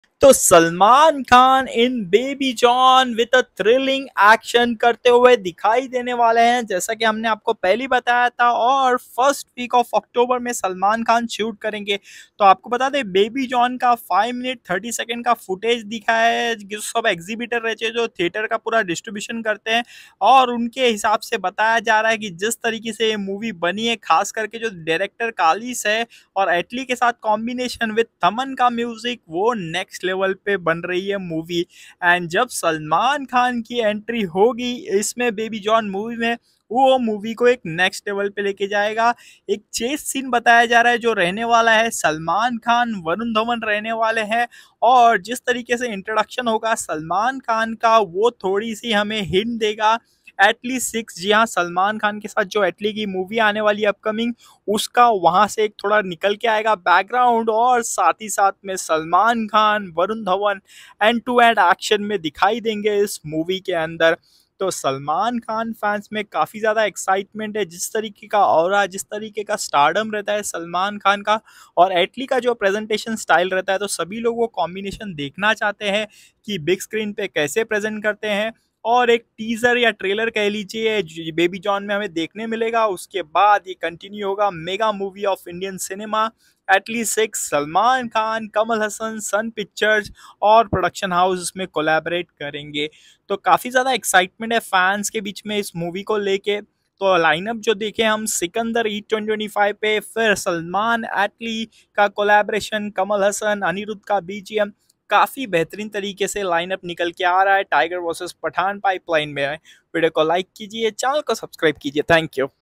Salman Khan Action Cameo for Baby John Directed by Atlee Shooting Details🔥
Salman Khan to shoot for smashing action cameo for Baby John movie from this date directed by Atlee🔥